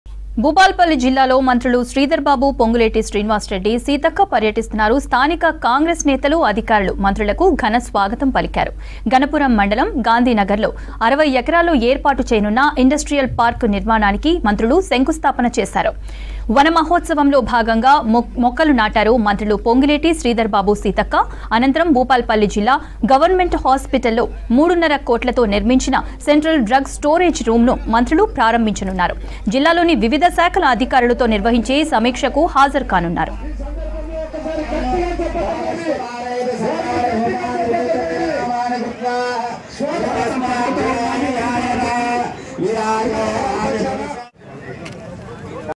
Seethakka, Ponguleti, Sridhar Babu To Lay Foundation Stone For Industrial Park | Bhupalpally | V6
Seethakka, Ponguleti, Sridhar Babu To Lay Foundation Stone For Industrial Park | Bhupalpally | V6 News